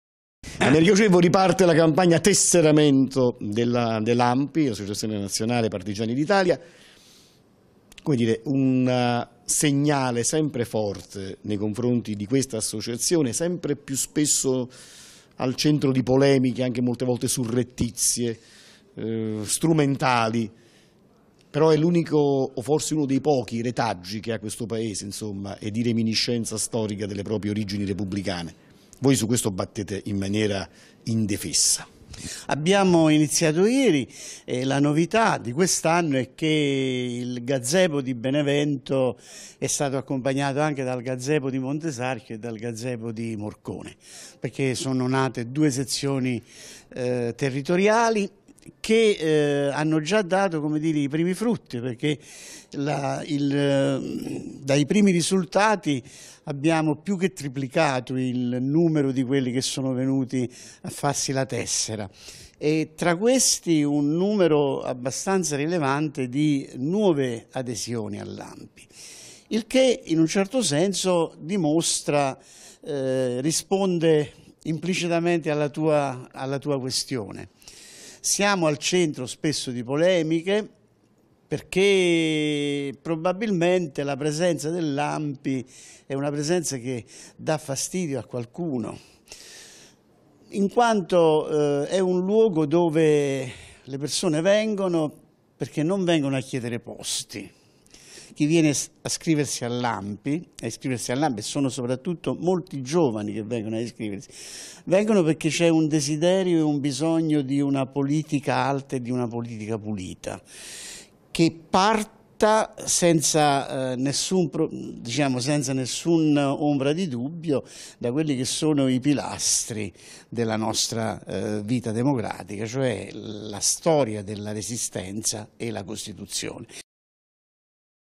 Parte il tesseramento ANPI 2020
Parte il tesseramento a Benevento. Ai microfoni di LabTv Amerigo Ciervo Segr. Provinciale ANPI Benevento